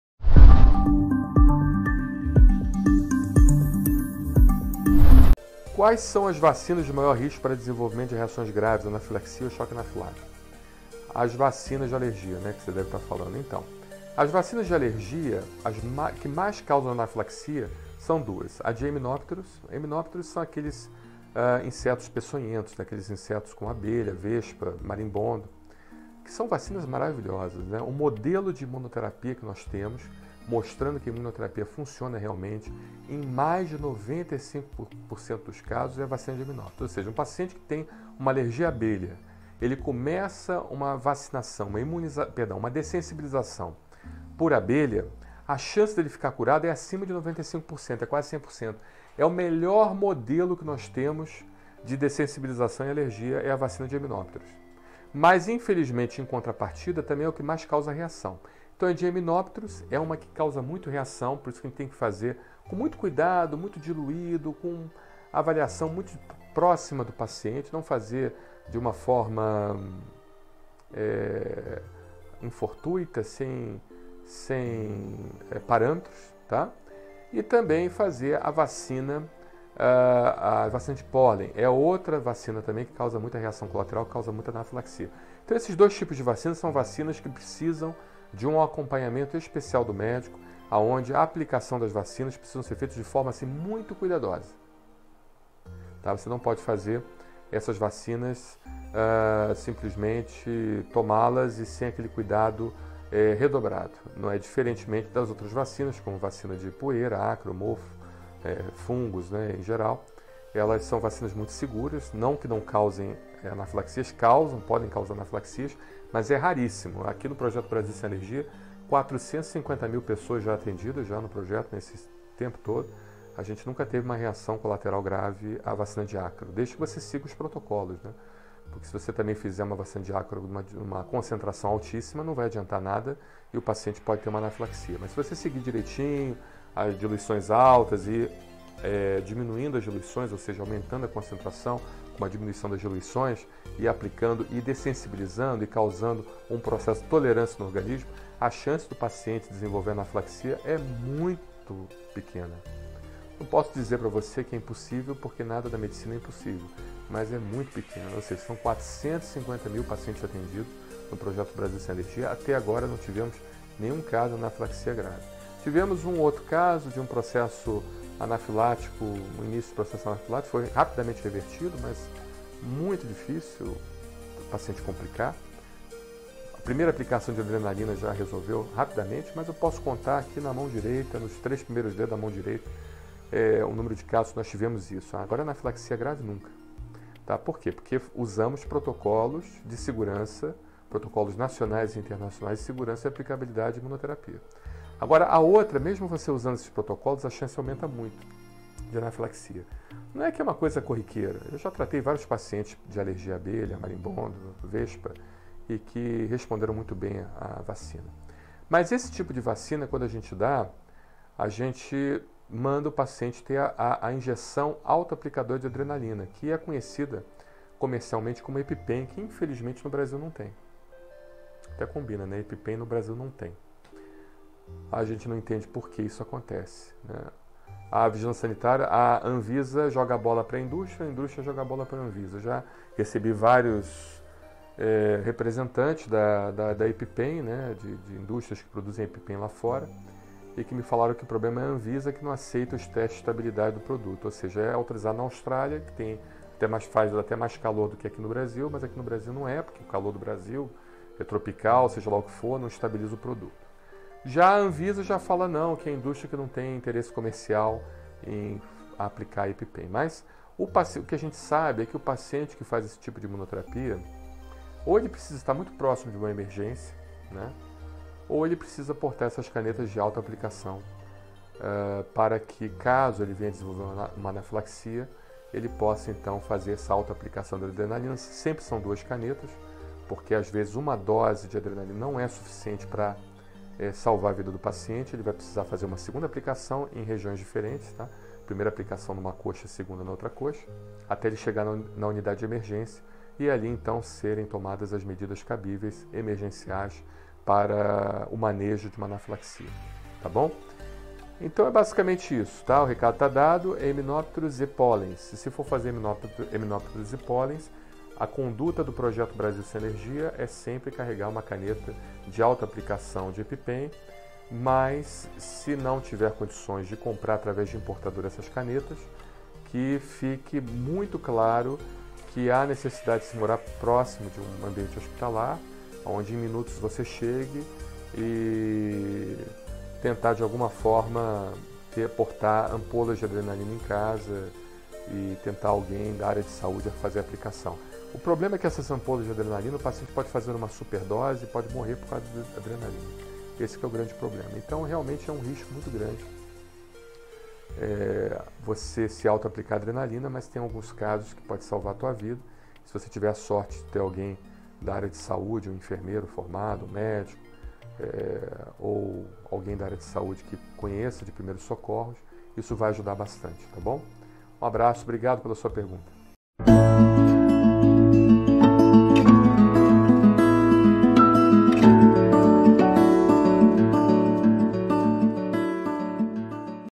Quais as vacinas de maior risco para desenvolver reações graves como choque anafilático?
Quais são as vacinas de maior risco para o desenvolvimento reações graves anafilaxia ou choque anafilático?